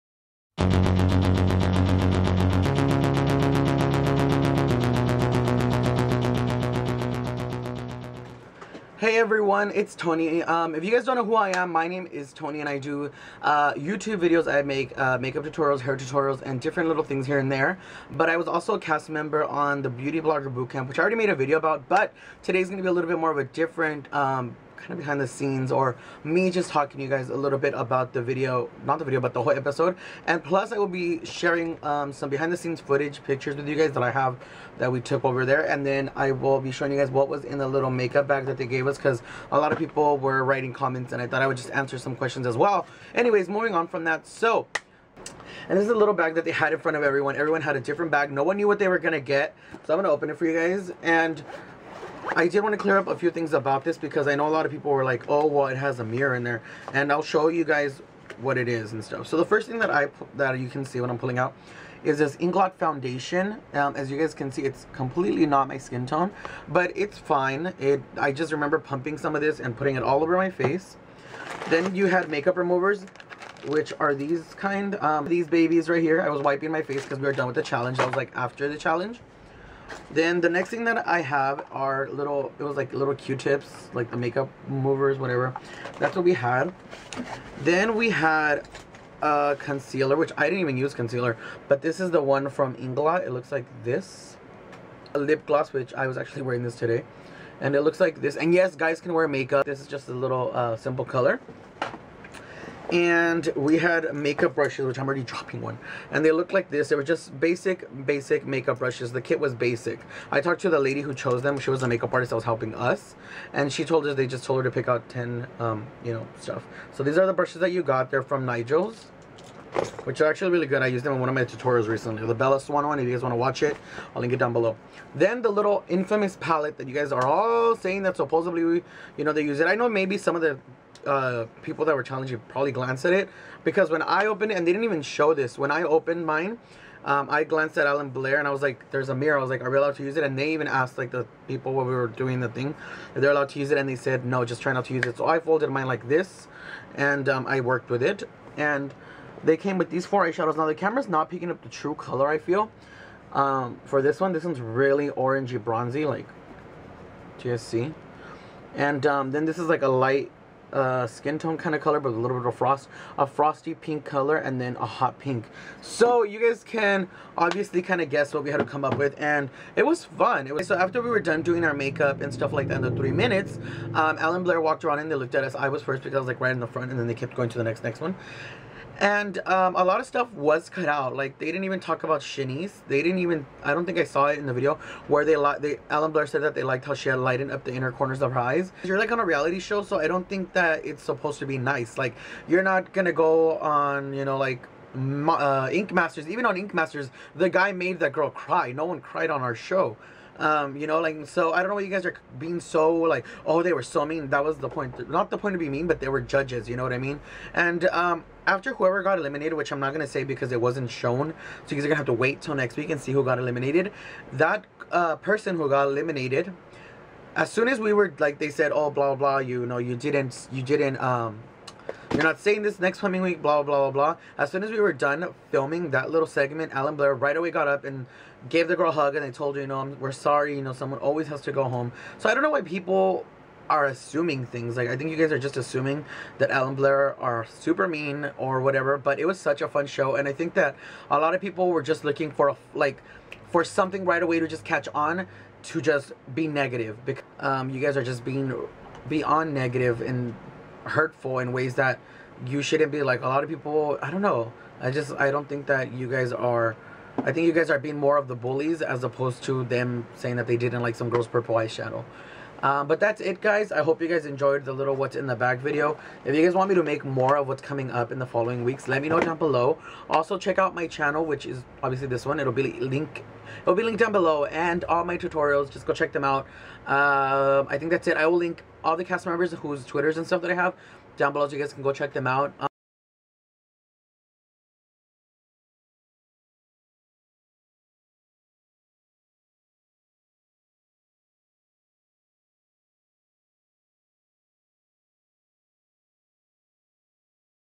Beauty Vlogger Bootcamp Wake-up No Make-up WHATS IN MY MAKEUP BAG!
READ
Hey everyone i hope you enjoy this video i just had see way to many questions on whats been in this bag.
so i thought i would show you guys. i hope you like.